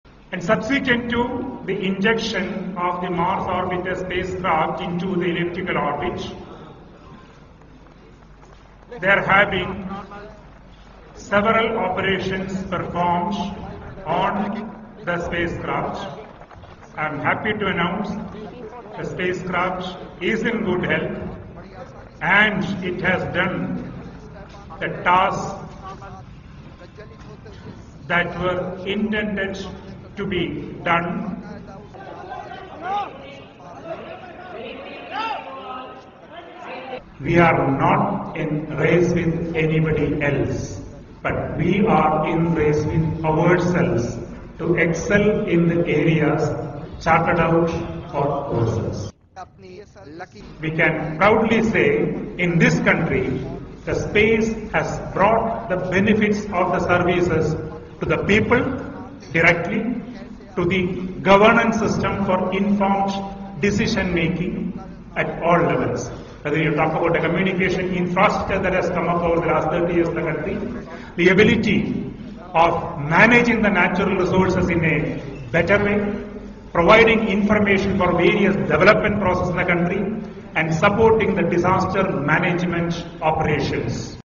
India Space Agency Says Mars Mission Successful, Spacecraft In Good Health
India on Tuesday (November 5) successfully launched its first rocket to Mars, aiming to put a satellite in orbit around the red planet at a lower cost than previous missions and potentially positioning the emerging Asian nation as a budget player in the global space race.

The Mars Orbiter Mission blasted off from Satish Dhawan Space Centre in India's Sriharikota with the satellite scheduled to start orbiting Mars by September, searching for methane and signs of minerals.

The Chief of India's Space Research Organisation (ISRO), K. Radhakrishnan said all went according to plan:

"Subsequent to the injection of the Mars Orbiter spacecraft into the elliptical orbit, there have been several operations performed on the spacecraft. I am happy to announce that the spacecraft is in good health and it has done the tasks that were intended to be done," said Radhakrishnan.

"We are not in race with anybody else but we are in race with ourselves to excel in the areas chartered out for ourselves," he added.

Only the United States, Europe, and Russia have sent probes that have orbited or landed on Mars. Probes to Mars have a high failure rate and a success will be a boost for national pride, especially after a similar mission by China failed to leave Earth's orbit in 2011.

India's ties with its neighbour are marked as much by competition as cooperation. Government scientists deny any space race, but analysts say India has stepped up its programme because of concerns about China's civilian and military space technology.

The probe's 4.5 billion rupee price tag is a fraction of the cost of NASA's (National Aeronautics and Space Administration) MAVEN (Mars Atmosphere and Volatile Evolution) mission, also due to launch in November. Analysts say India could capture more of the $304 billion global space market with its low-cost technology.

Listing the benefits of the mission, Radhakrishnan said that India's space technology has assisted in disaster management operations and communication technology.

"We can proudly say in this country the space has brought the benefits of the services to the people directly to the governance system for informed decision making at all levels, whether you talk about communication infrastructure that has come in the last 12 years in the country. The ability of managing the natural resources in a better way, providing information of various development projects in the country and supporting the disaster management operations," he added.

The Mars mission is considerably cheaper than some of India's more lavish spending schemes, including a $340 million plan to build the world's largest statue in the state of Gujarat, including surrounding infrastructure.

Even so, it has drawn criticism in a country suffering from high levels of poverty, malnutrition and power shortages and experiencing its worst slowdown in growth in ten years.

India's space programme began 50 years ago and developed rapidly after Western powers imposed sanctions in response to a nuclear weapons test in 1974, spurring scientists to build advanced rocket technology. Five years ago, its Chandrayaan probe landed on the moon and found evidence of water.

The relative prowess in space contrasts with poor results developing fighter jets by India's state-run companies.

The Mars Orbiter Mission plans to search for methane in the Martian atmosphere, the chemical strongly tied to life on Earth. Recent measurements made by NASA's rover, Curiosity, show only trace amounts of it on Mars.

India's mission will also study Martian surface features and mineral composition.